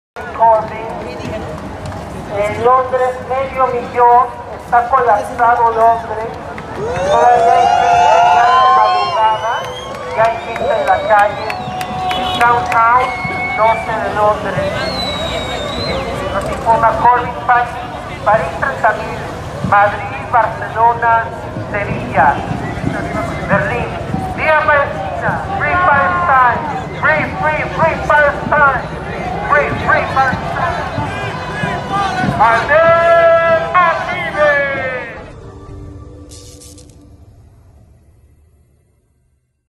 Mexico City stands with Palestine!
A big rally in Mexico City to show solidarity with Palestine and to demand an immediate ceasefire in Gaza now!